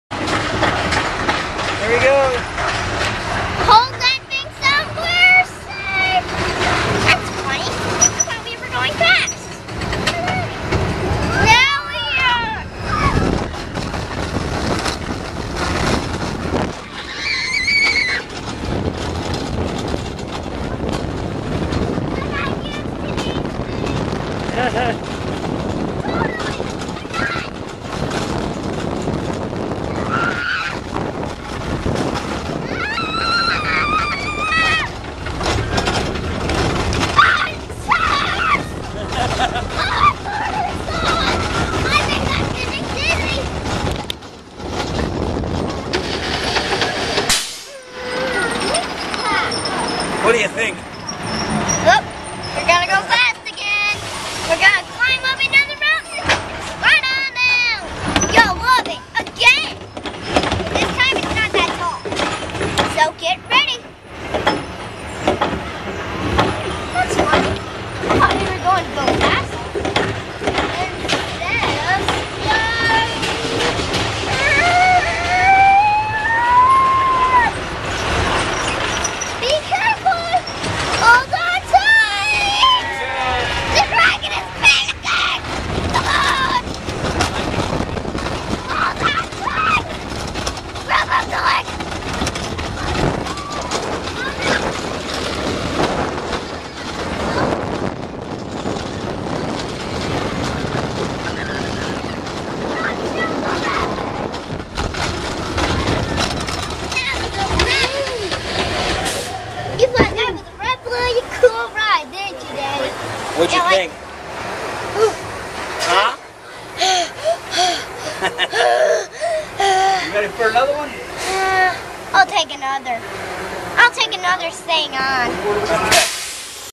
Dominic vs. The Iron Dragon [Cedar Point]
This is too funny.  My 6-year-old son during his second ride ever on a real roller coaster, the Iron Dragon at Cedar Point.  He's a born thrill-seeker.  He'd already ridden once, so felt qualified to narrate the action.  He's the man.  Recorded on August 18, 2010 using a Flip Video camcorder.